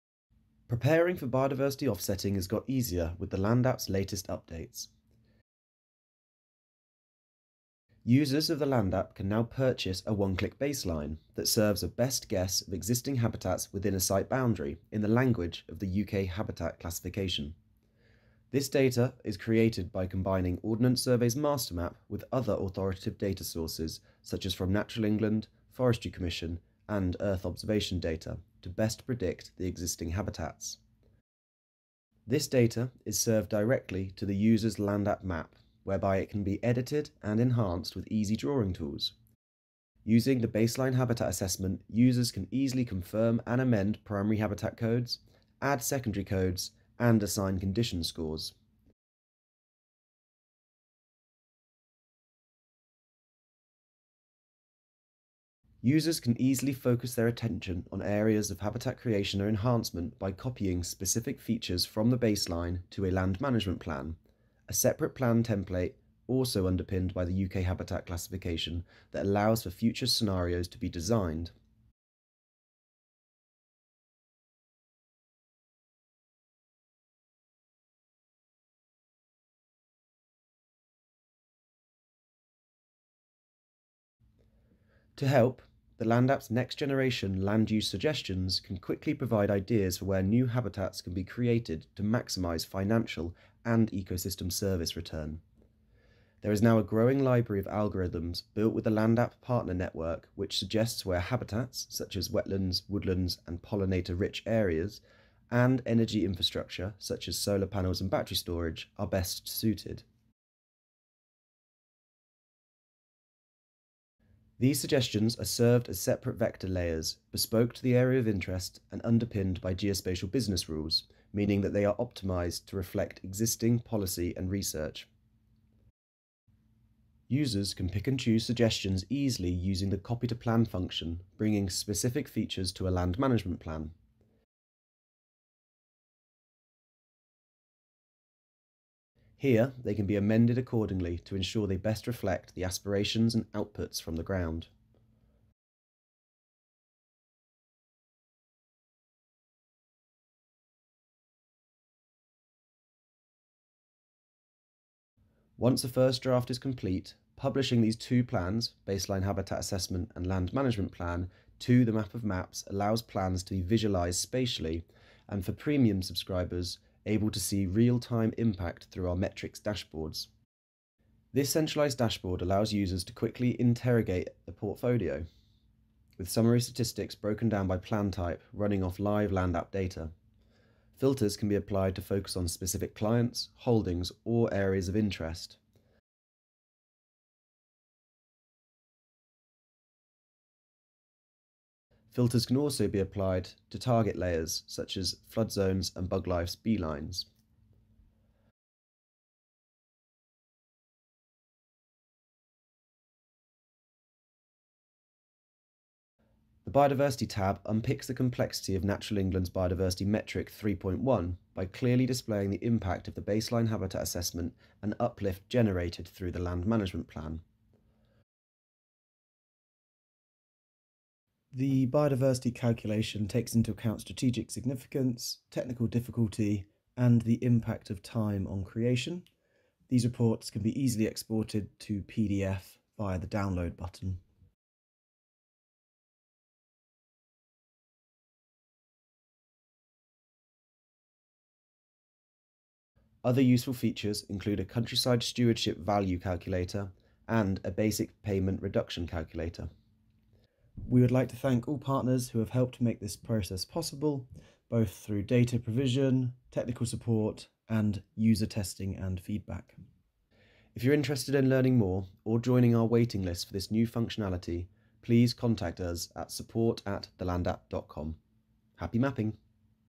Demo Video - Biodiversity Net Gain workflow - with Land App
Version 1: Our latest release allows users to quickly prepare for Biodiversity Net Gain:

- Purchase a "UK Habitat Best Guess Baseline" (the best guess of existing habitats that can be edited and enhanced), generate a "Land Management Plan" (outlining areas for creation or enhancement, including "Land-use Ideas" from Land App partners) and then calculate the impact as Biodiversity Units using our Metric's Dashboard.
- Video created in January 2023. Subscription and In-App purchases are required.